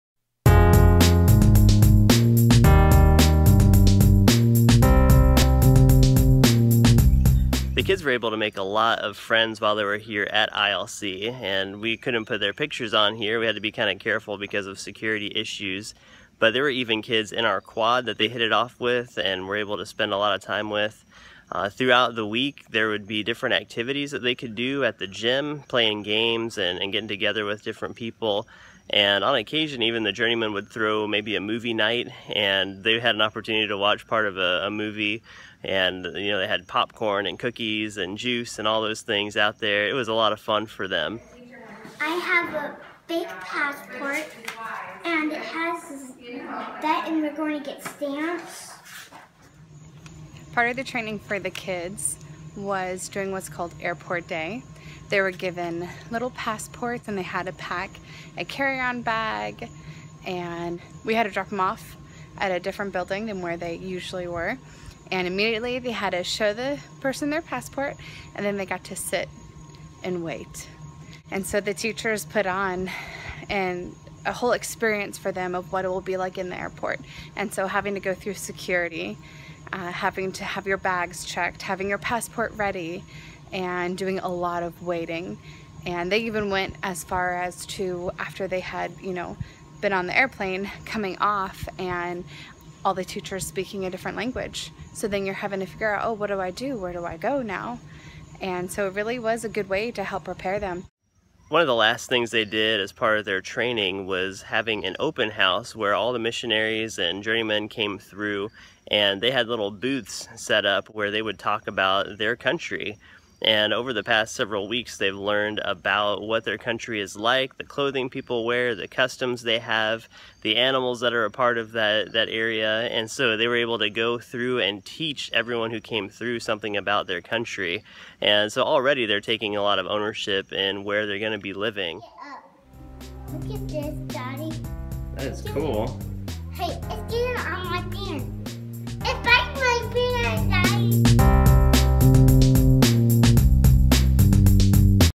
FPO - The Kid's Training
FPO (Field Personnel Training) has ended, but we wanted to give you a more comprehensive look into what our time was like with our kids. The teachers at ILC (International Learning Center) are amazing! They do such a great job getting children prepared for living over seas.